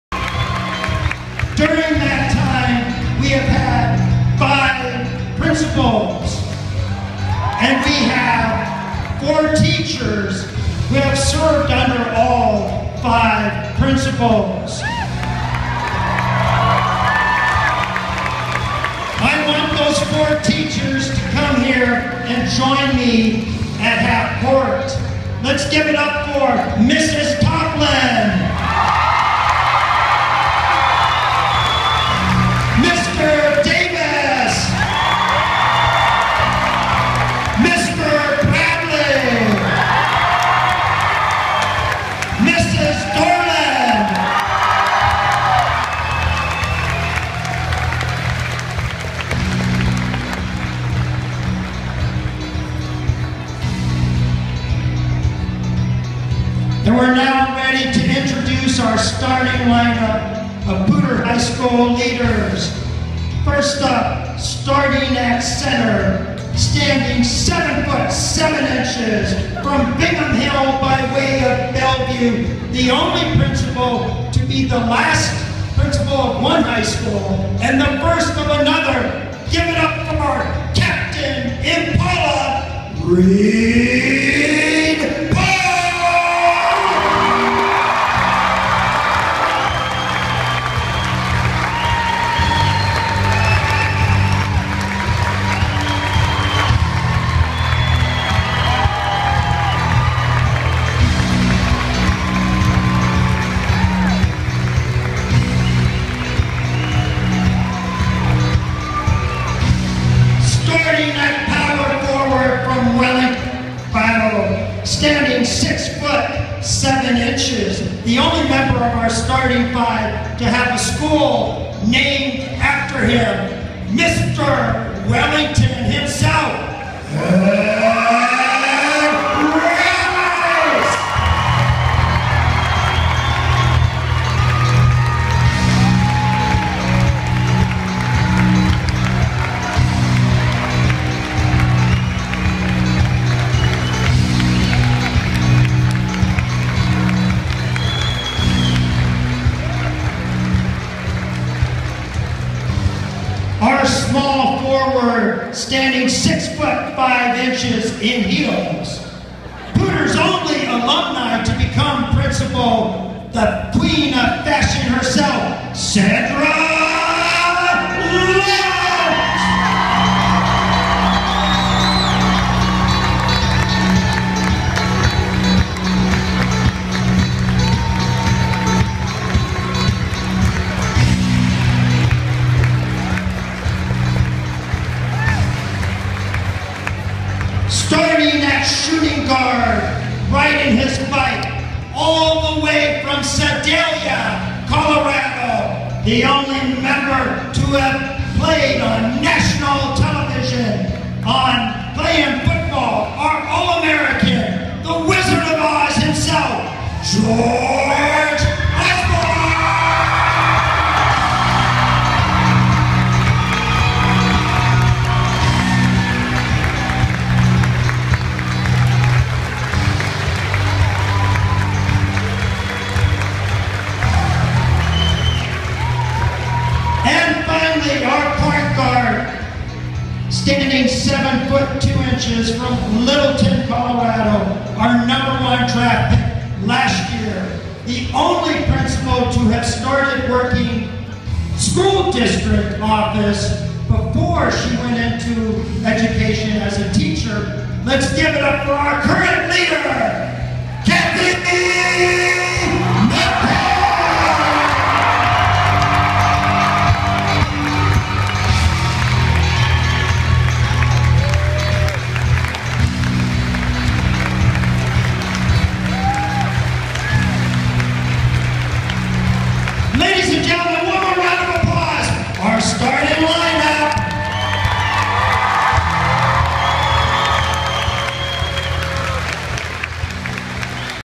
Russ Brown Introducing The 5 Principals of Poudre High School 50th Homecoming Anniversary
Russ Brown Introducing The 5 Principals of Poudre High School 50th Home Coming Anniversary